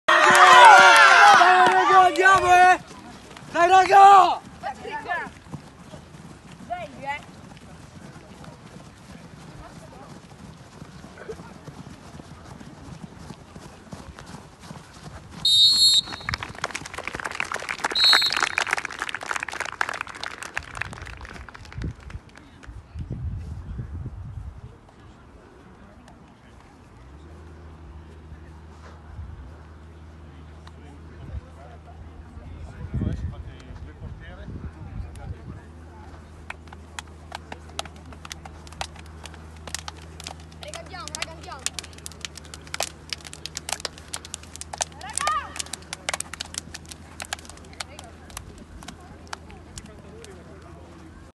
Under 13, 3^ Giornata Parma-Sassuolo, ingresso in campo, cerimoniale d'inizio
~ Under 13, 3^ Giornata Parma-Sassuolo, ingresso squadre in campo, cerimoniale d'inizio
Campo in sintetico del Centro Sportivo di Collecchio (PR), Sabato 29 Settembre 2019